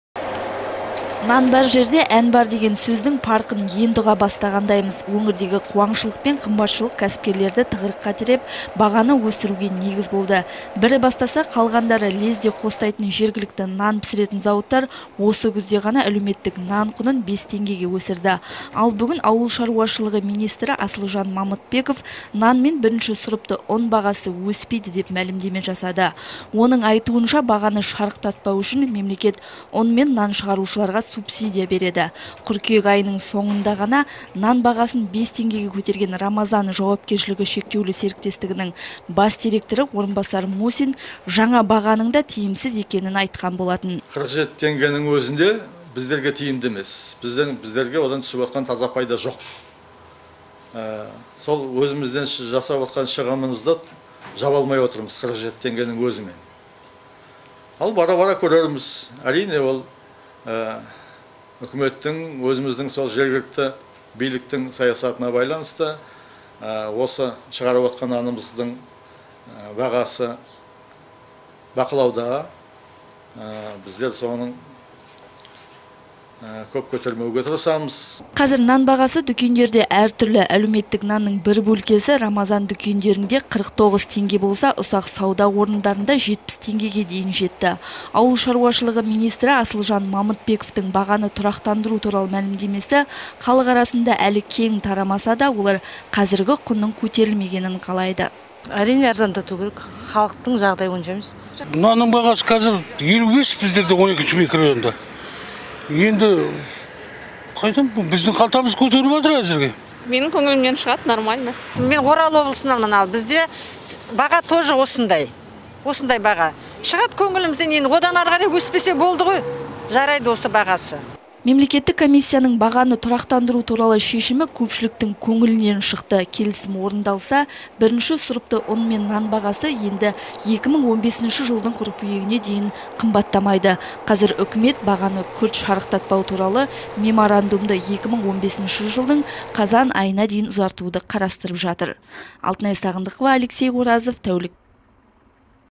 Нан бағасы келесі күзге дейін өспейді
Елімізде нан бағасы келер жылдың қыркүйегіне дейін өспейді. Бұл мәлімдемені бүгін ауыл шаруашылығы министрі Асылжан Мамытбеков жасады. Министр мемлекеттік комиссияның осы шешімін парламент мәжілісінде хабарлады. Ол бағаны тұрақты ұстап тұру үшін арнайы субсидия бөлінетінін де тілге тиек етті. Ал қаламызда қыркүйекте ғана әлеуметтік нан бағасы 5 теңгеге қымбаттаған болатын. Өңірдегі қуаңшылық пен қымбатшылық кәсіпкерлерді тығырыққа тіреп, бағаны өсіруге негіз болды. Ауыл шаруашылығы министрі Асылжан Мамытбековтің  айтуынша бағаны шарықтатпау үшін мемлекет ұн мен нан шығарушыларға  субсидия береді. Қазір нан бағасы дүкендерде әртүрлі. Әлеуметтік нанның бір бөлкесі "Рамазан" дүкендерінде 49 теңге болса, ұсақ сауда орындарында 70 теңгеге дейін жетті. Ауыл шаруашылығы министрі Асылжан Мамытбековтың бағаны тұрақтандыру туралы мәлімдемесі халық арасына әлі кең тарамаса да олар қазіргі құнның көтерілмегенін қалайды. Келісім орындалса, бірінші сұрыпты ұн мен нан бағасы енді 2015 жылдың қыркүйегіне дейін қымбаттамайды. Қазір үкімет бағаны күрт шарықтатпау туралы меморандумды 2015 жылдың қазан айына дейін ұзартуды қарастырып жатыр.